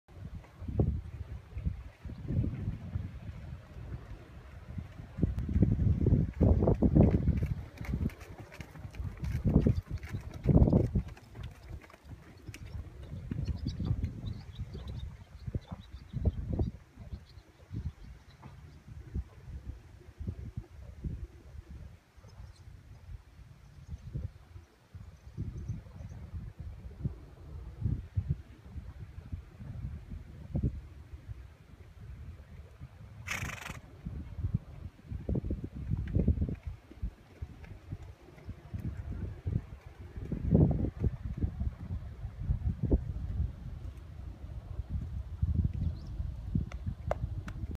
Neve and Joe flatwork summer 2015
Neve and Joe's first summer together practising flatwork